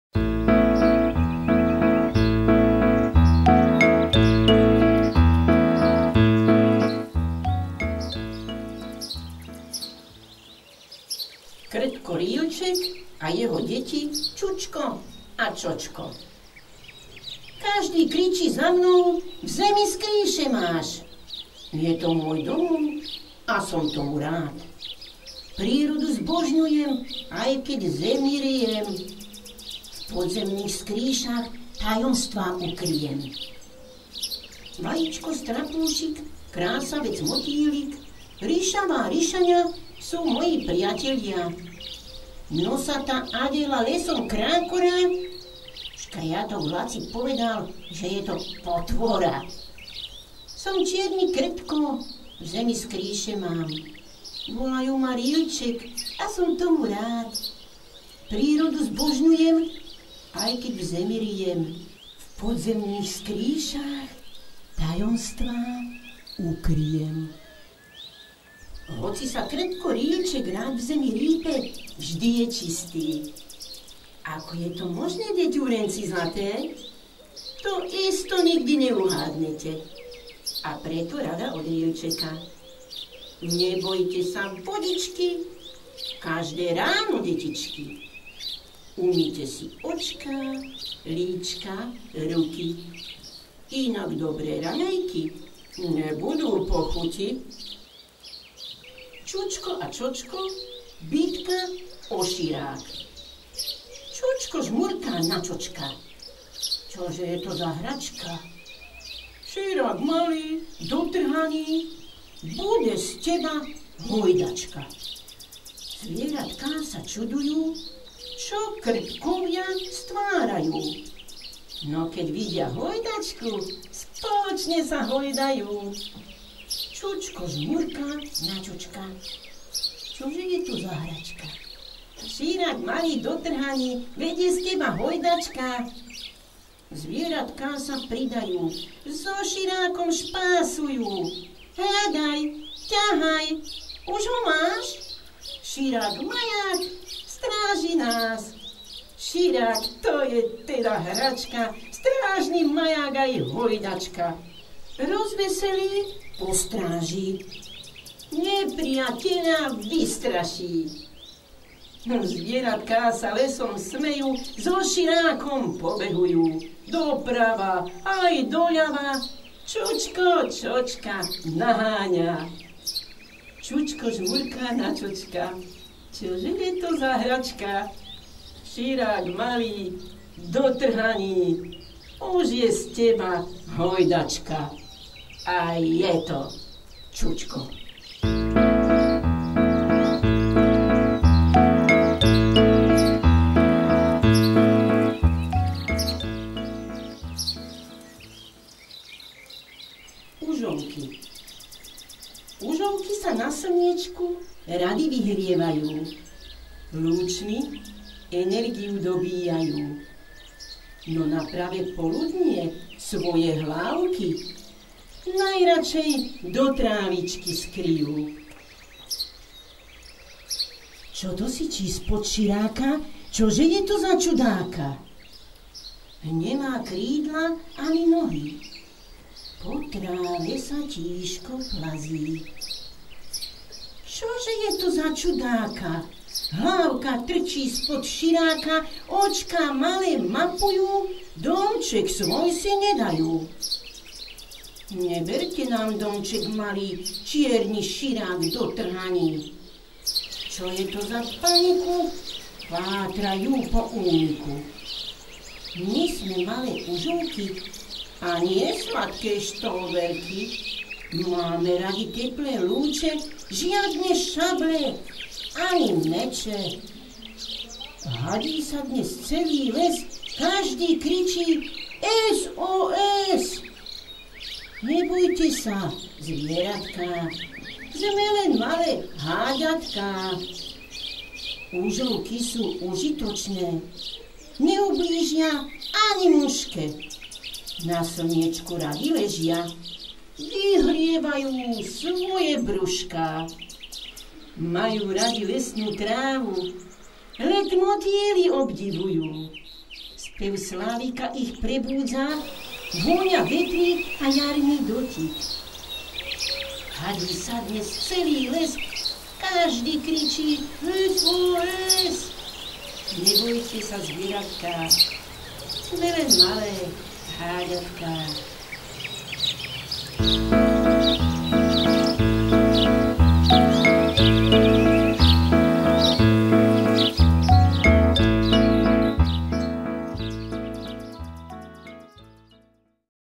2.KRTKOVIA aj užovky básne, rozprávky, príroda aj rady, čakajú na vaše deti
Príroda, básničky, spev vtáčikov,
 aj dobré rady, čakajú na vaše deti.